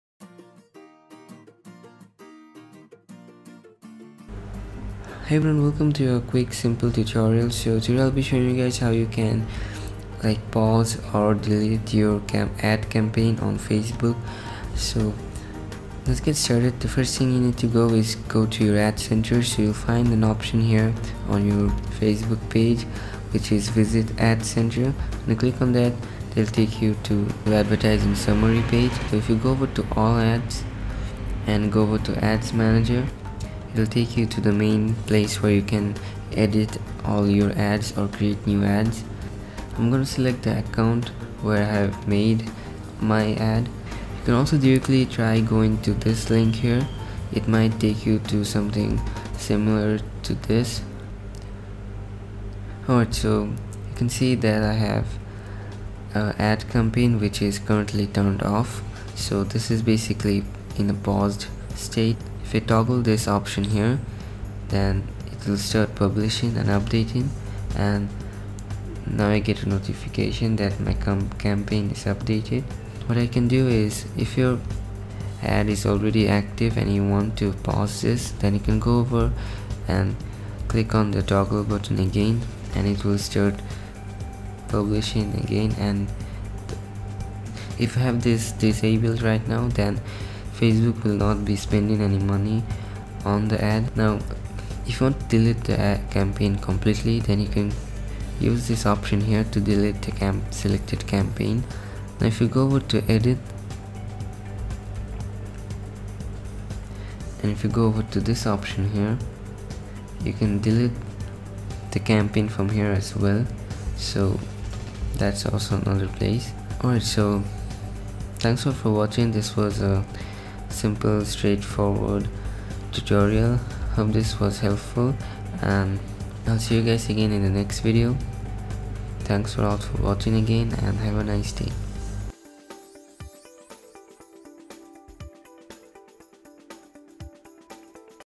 How to Pause, Delete Stop Ads - Facebook Advertising
This video shows the easy option that controls the ad active and in-active status.